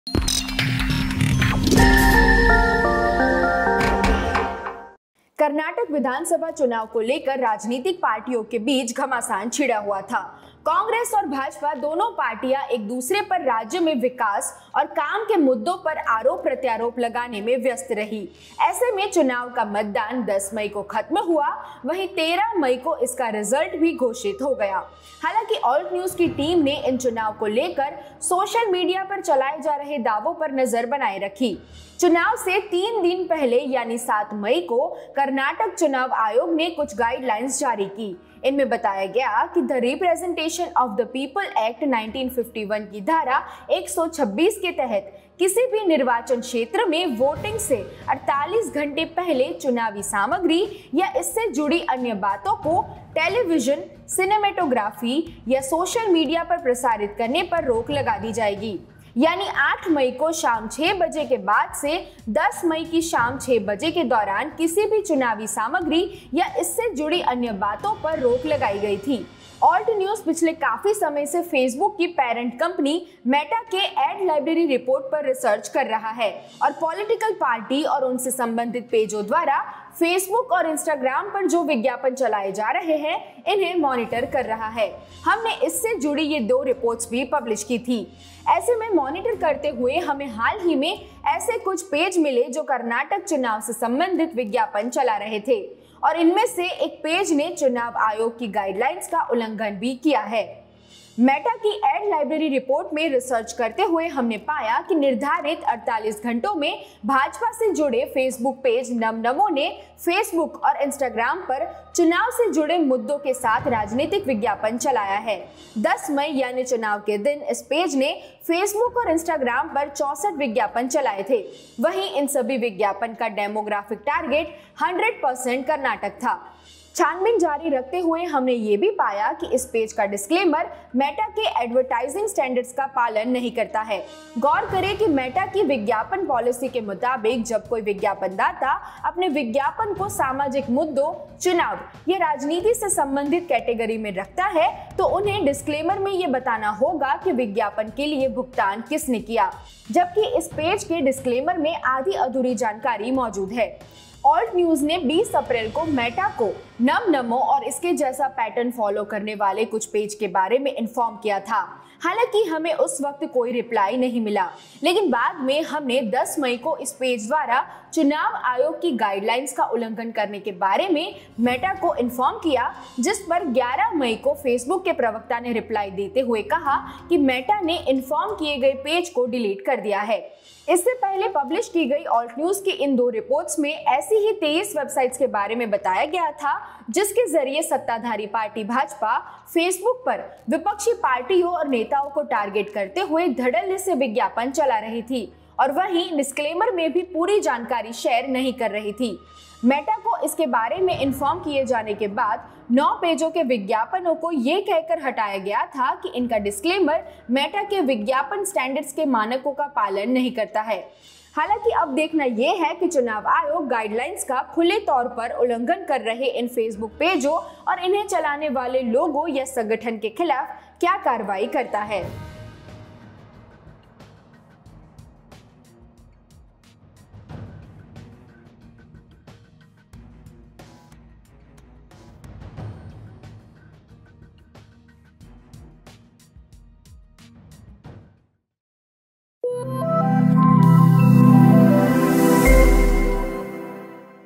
Karnataka election: चुनाव आयोग की गाइडलाइंस का उल्लंघन करते हुए पाया गया BJP से जुड़ा फ़ेसबुक पेज
कर्नाटक चुनाव के दिन तक बीजेपी से जुड़ा एक फ़ेसबुक पेज प्रचार करता हुआ पाया गया. ये चुनाव आयोग की गाइडलाइंस का उल्लंघन है. ऑल्ट न्यूज़ ने जांच कर इस मामले के संदर्भ में मेटा को सूचित किया जिसके बाद इस पेज को प्लेटफ़ॉर्म से हटा दिया गया. देखिये मामले की पूरी रिपोर्ट